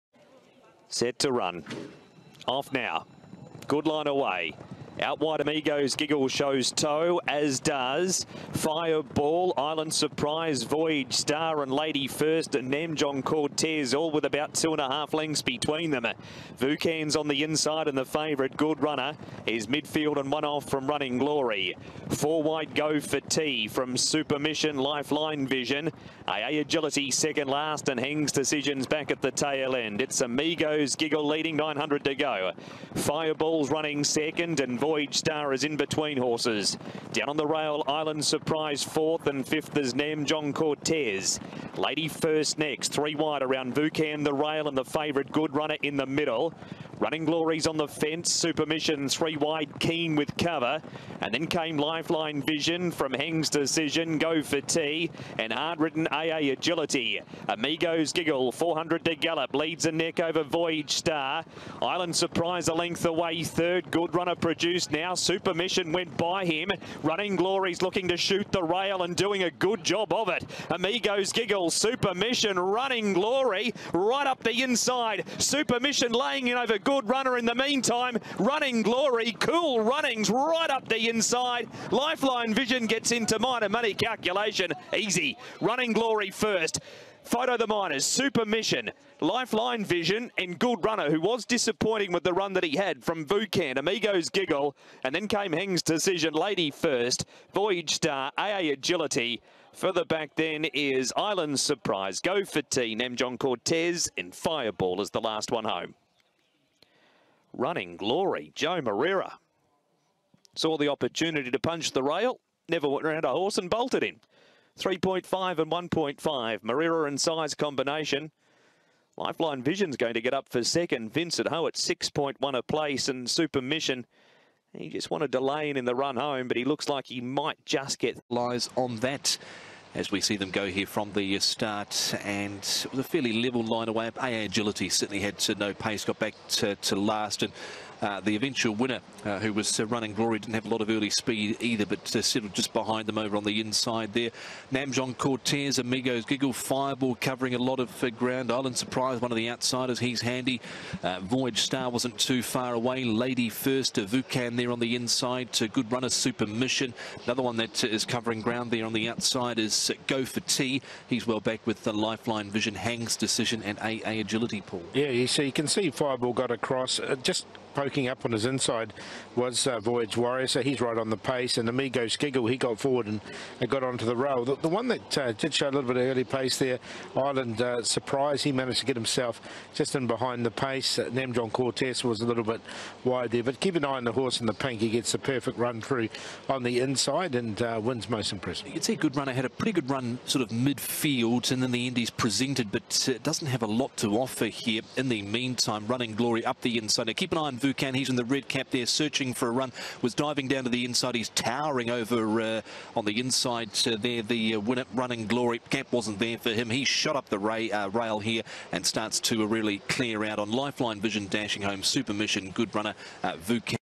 9/1/2022 (325) Running Glory - J Moreira
Class 4 1400m  Sha Tin (C Course)
Trainer: J Size
2nd: Lifeline Vision
3rd: Super Mission